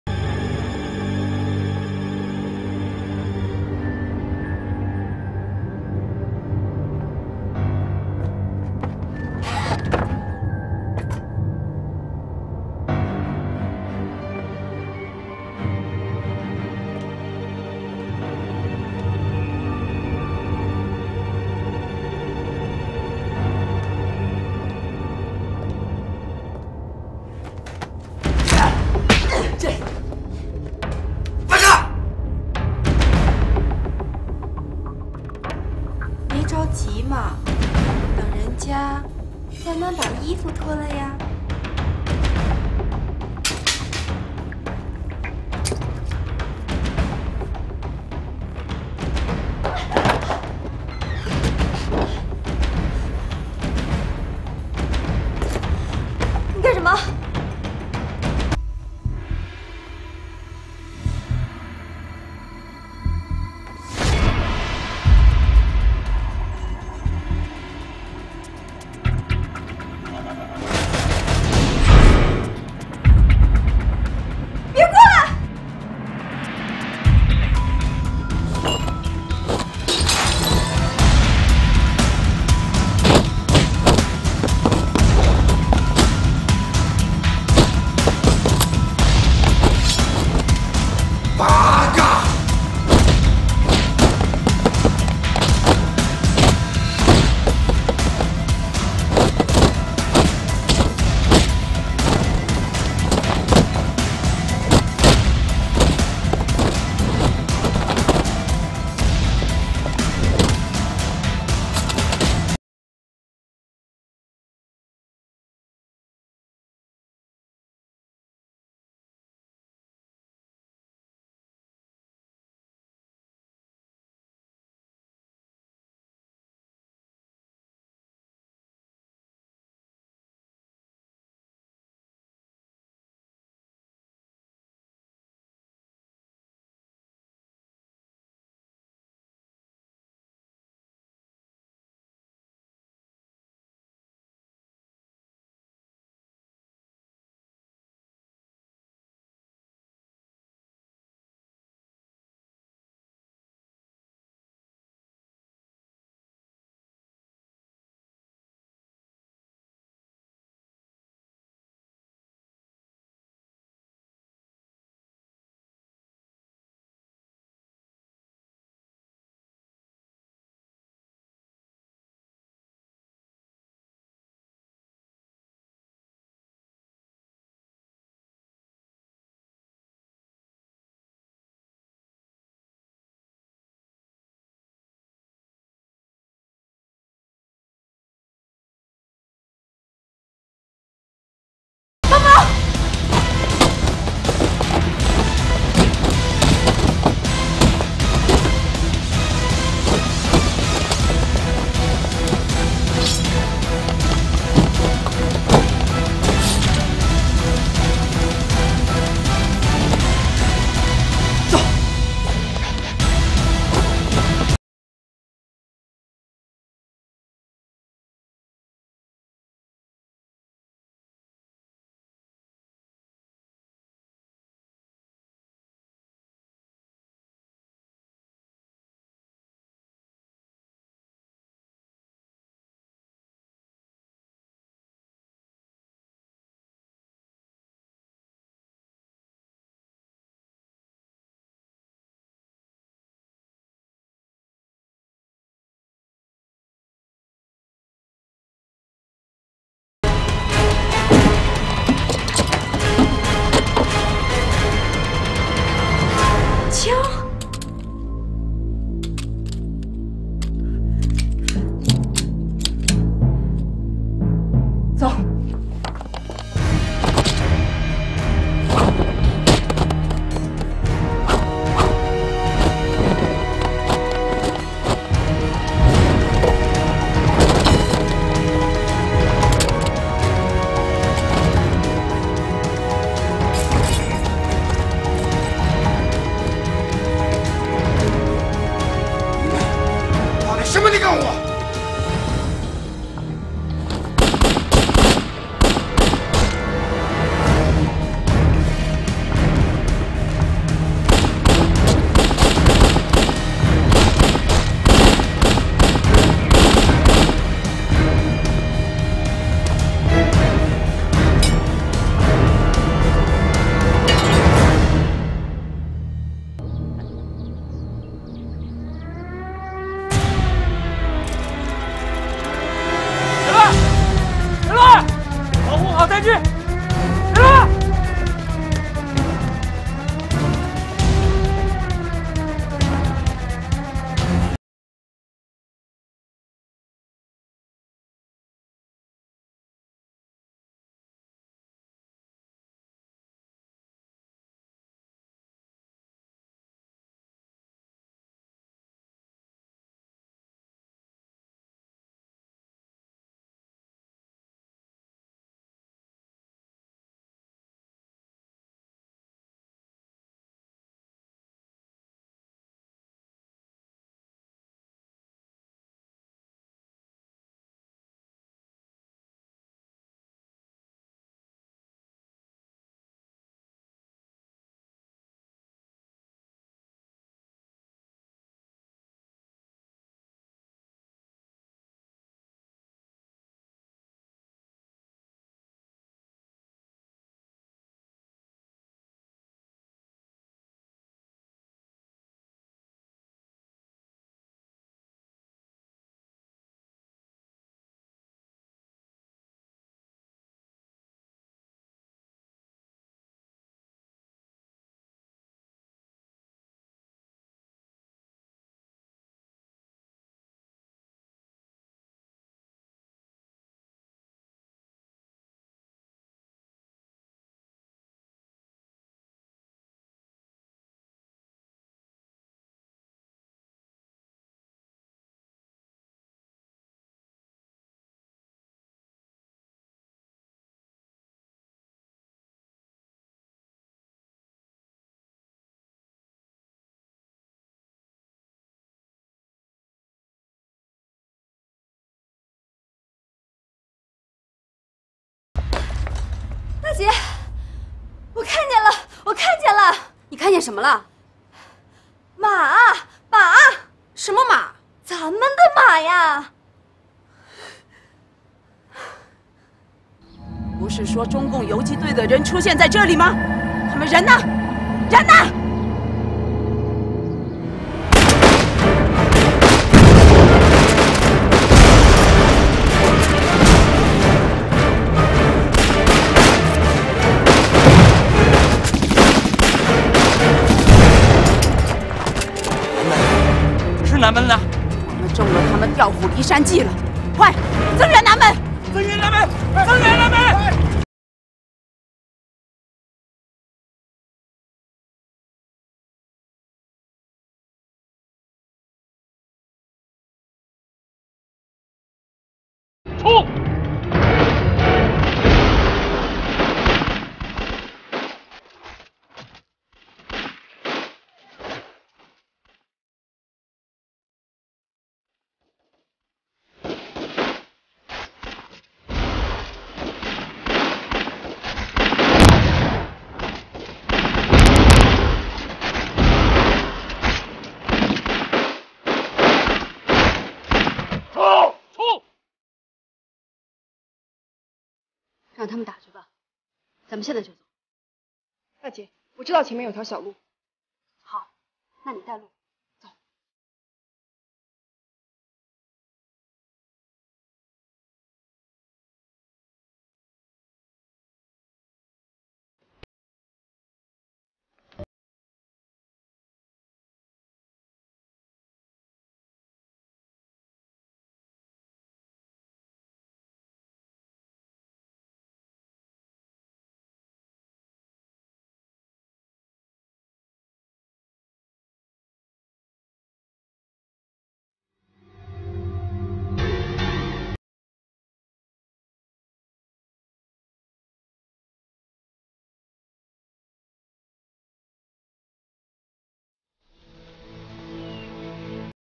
女八路打扮成風塵女子潛伏在日軍內部，利用女人優勢，刺殺日軍高官 ⚔️ 抗日
FHTQ